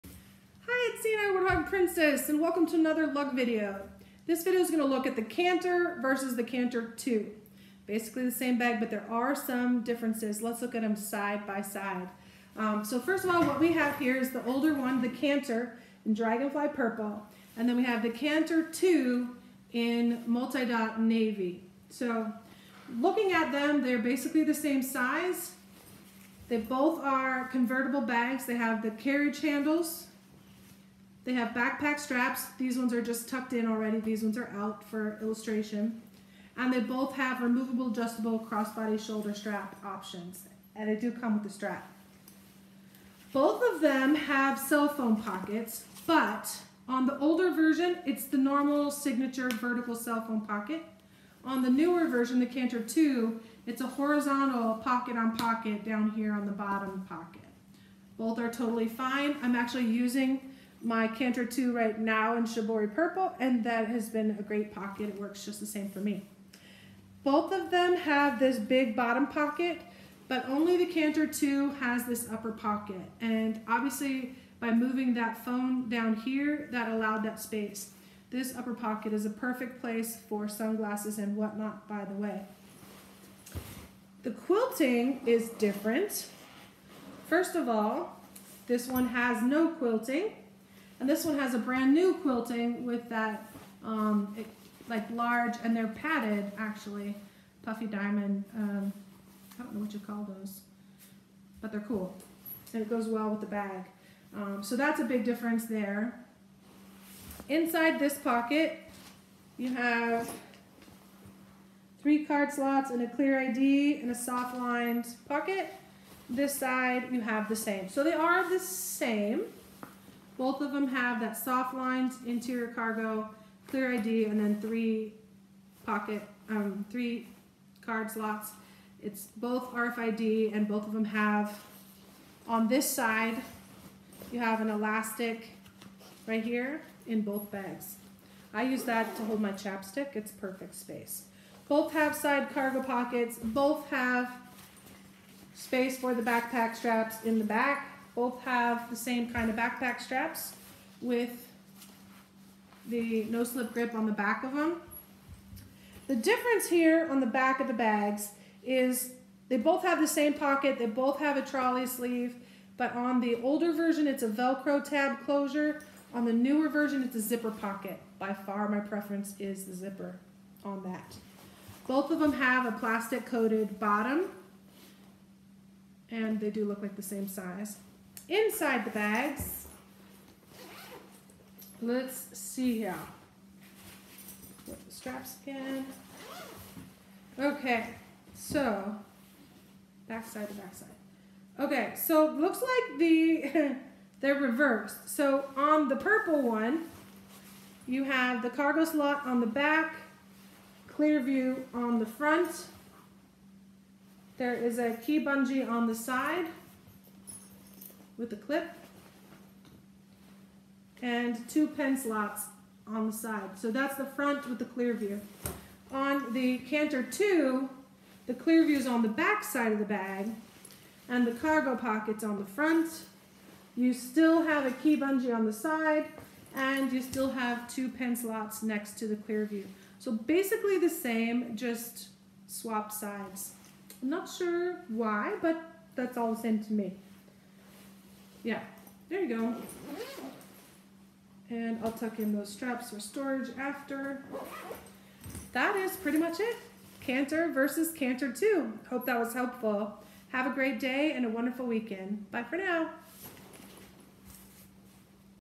Comparison & Review — Lug Canter vs Canter 2
This video is a comparison and review of the Lug Canter and specifically takes a detailed look at the similarities and differences between the Canter and the NEW Canter 2. The bags shown in this video are the Canter (Dragonfly Purple) and the Canter 2 (Multi Dot Navy).